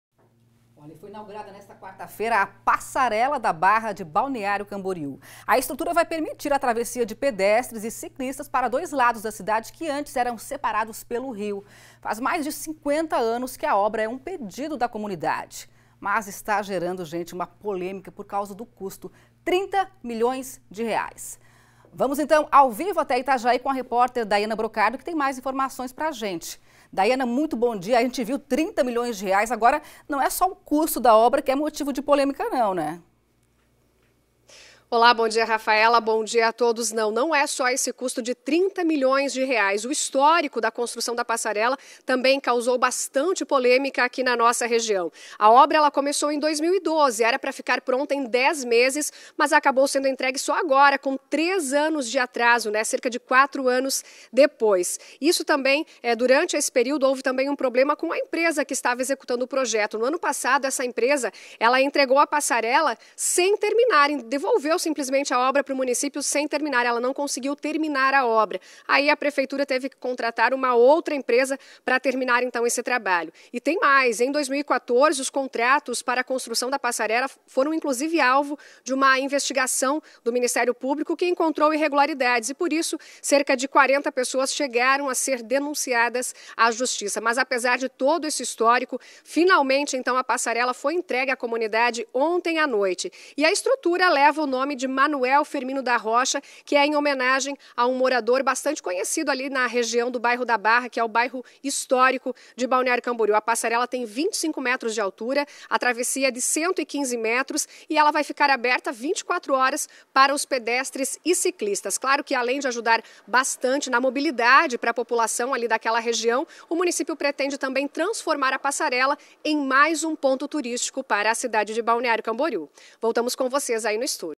Inauguração da Passarela da Barra gera polêmica em Balneário Camboriú
Foi inaugurada a Passarela da Barra em Balneário Camboriú. A estrutura vai permitir a travessia de pedestres e ciclistas para os dois lados da cidade que antes eram separados pelo rio.
A repórter Daiana Brocado da mais informações sobre a polêmica causada pela inauguração da obra.